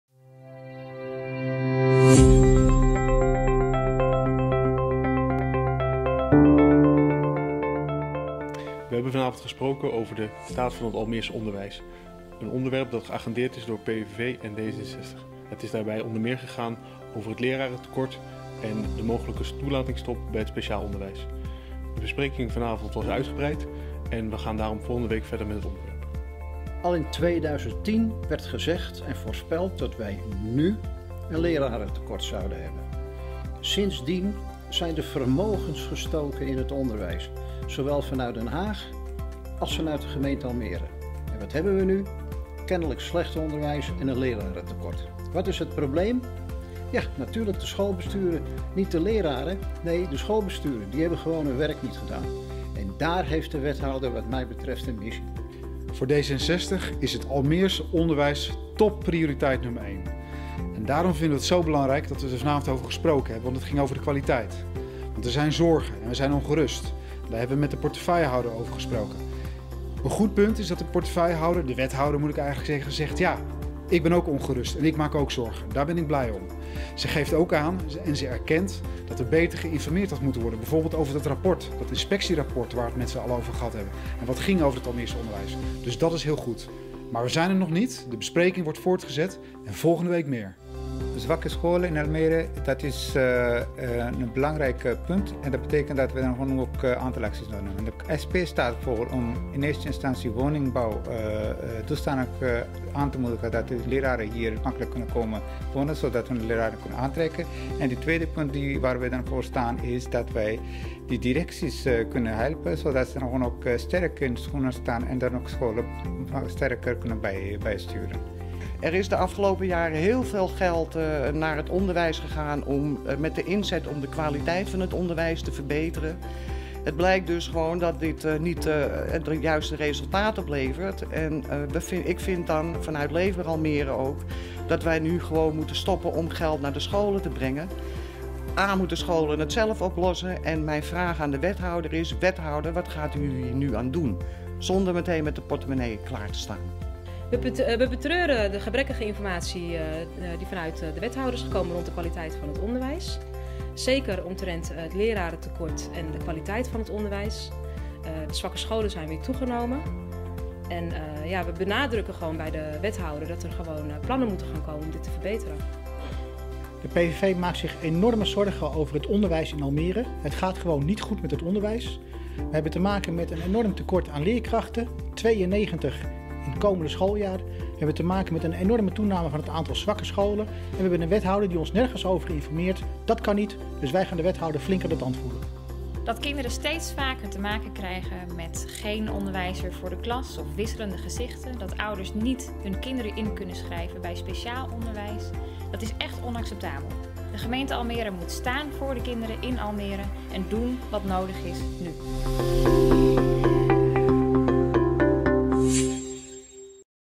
De staat van het onderwijs - Raad van Almere
Op camera spreken:
Jaap Steenkamer, raadslid en voorzitter van de vergadering
Johan de Leeuw, raadslid AP/OPA
Sven van den Burg, raadslid D66
Naeem Azhar, raadslid SP
Jannie Degenhardt, raadslid Leefbaar Almere
Zoubida Bouljhaf, fractieassistent GroenLinks
Chris Jansen, raadslid PVV
Leonie Vestering, raadslid Partij voor de Dieren

Ter vergadering waren aanwezig:
VVD
PvdA
PVV
D66
GroenLinks
SP
Leefbaar Almere
Partij voor de Dieren
ChristenUnie
CDA
AP/OPA